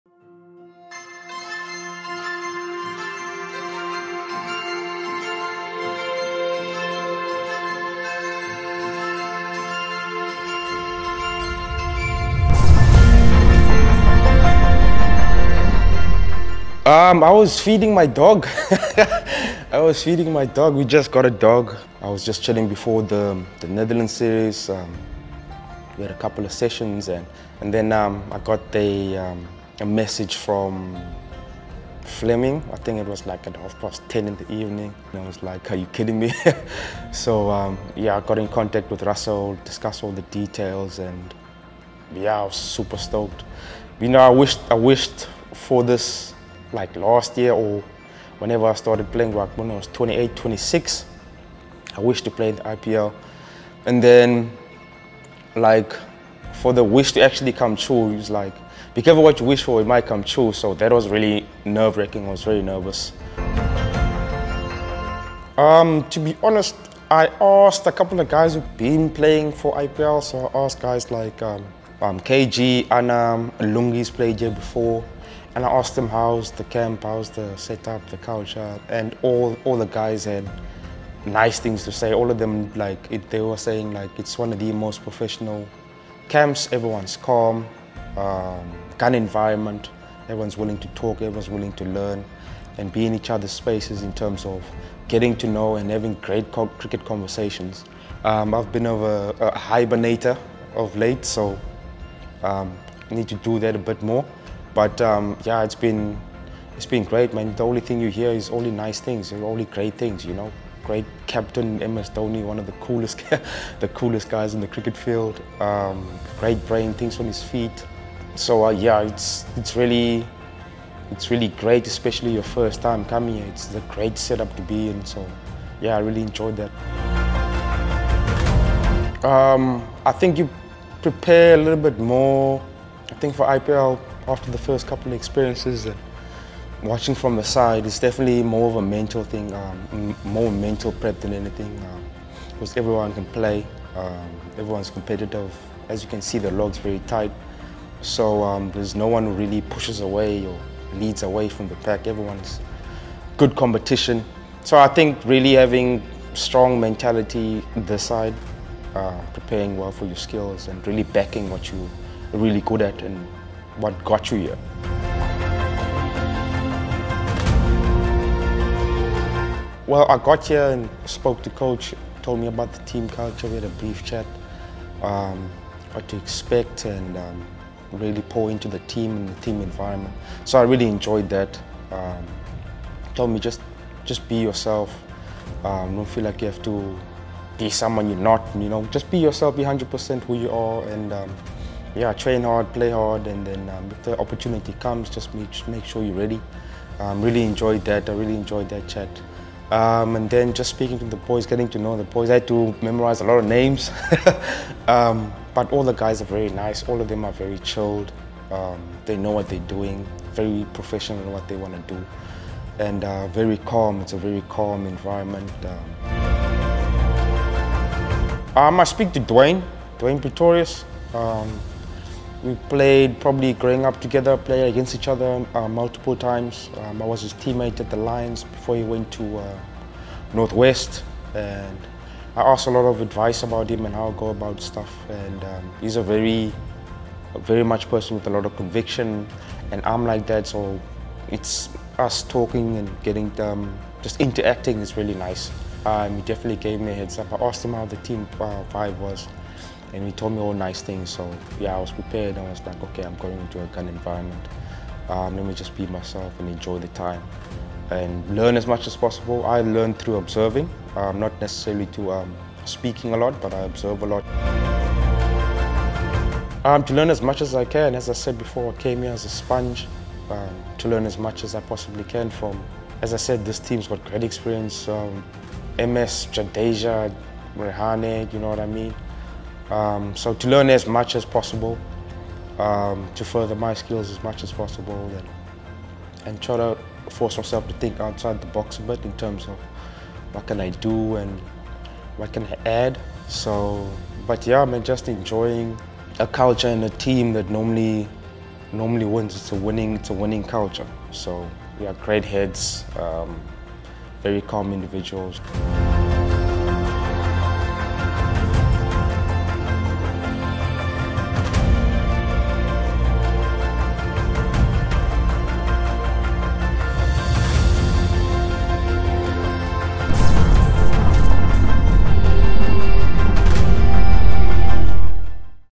First time in Yellove ft. Sisanda Magala
Magala narrates the story of his call up to represent the 🦁 and his first experience in Yellove 💛